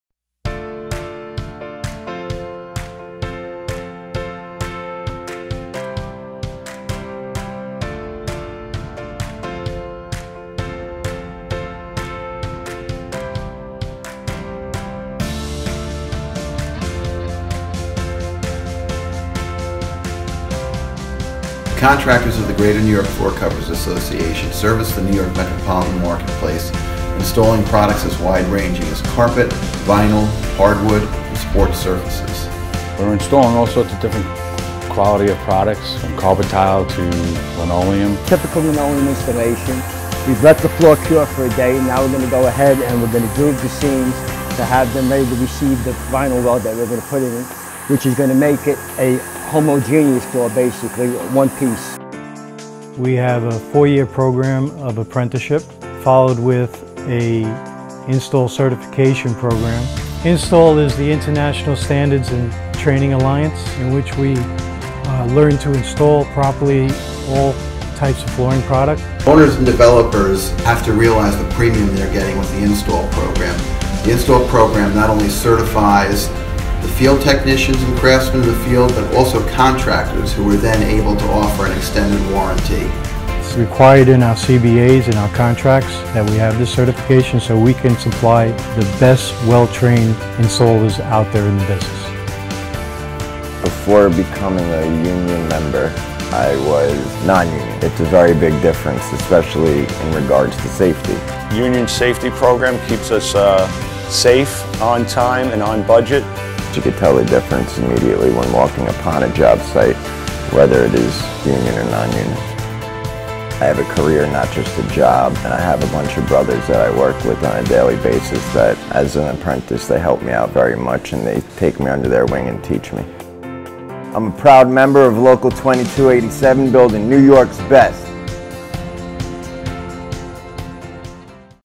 Spotlight on Resilient Floor Covering
Union floor covering carpenters and contractors are trained to meet the specifications of your project and the flooring product’s manufacturer. Because every flooring project should be installed with Quality and Performance to Stand On. Union carpenters and contractors are Building New York’s Best!